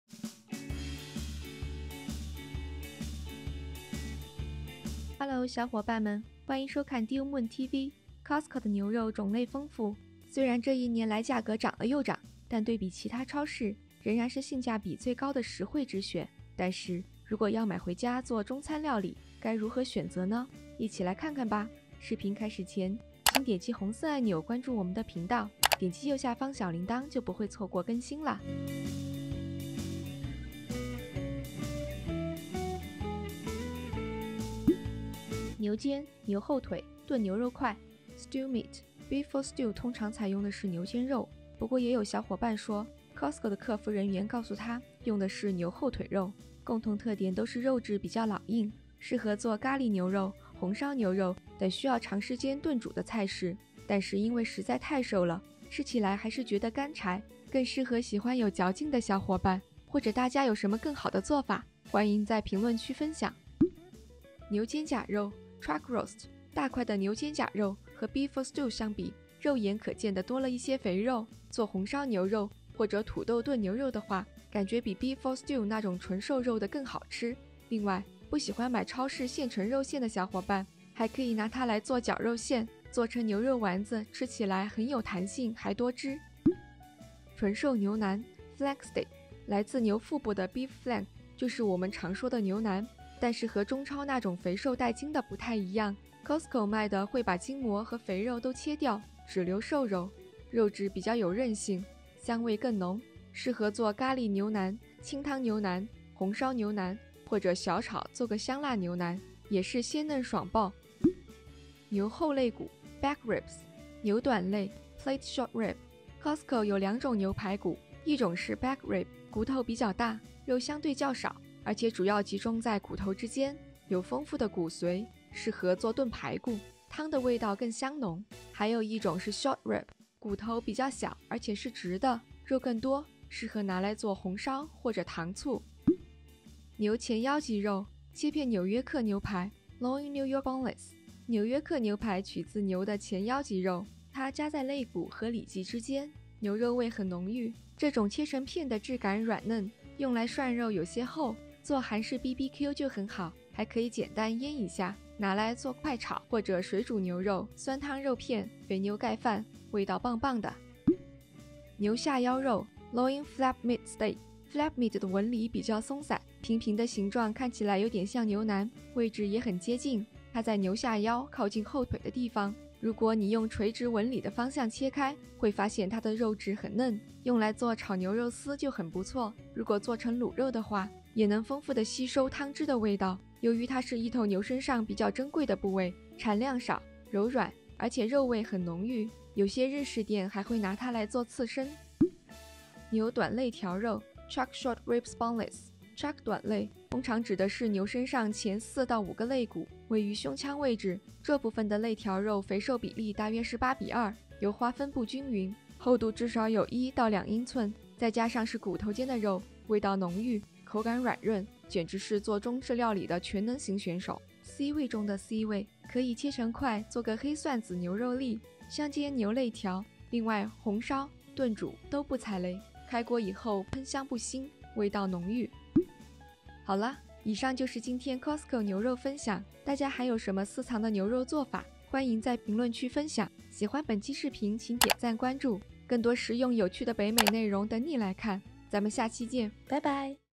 8款Costco牛肉盘点｜如何选择做中餐？这款全能型牛肉瞬间秒光～遇到必囤～
Costco的牛肉种类丰富，虽然这一年来价格涨了又涨，但对比其他超市，仍然是性价比最高的实惠之选。但是如果要买回家做中餐料理，应该如何选择呢，一起看看吧~

Beef Stew Meat USDA Choice
炖牛肉块（牛肩/牛后腿）
$5.19/lb

Chuck Roast Pot Roast Boneless USDA Choice
牛肩胛肉
$5.29/lb

Beef Flank Steak USDA Choice
纯瘦牛腩
$8.69/lb

Beef Back Ribs USDA Choice
牛后肋骨
$3.99/lb

Beef Plate Short Rib USDA Choice
牛短肋
$5.99/lb

Beef Loin New York Boneless USDA Choice
纽约客牛排（牛前腰脊肉）切片
$10.99/lb

Beef Loin Flap Meat Steak USDA Choice
牛下腰肉
$10.99/lb

Beef Chuck Short Ribs Boneless USDA Choice
牛短肋条肉
$15.99/lb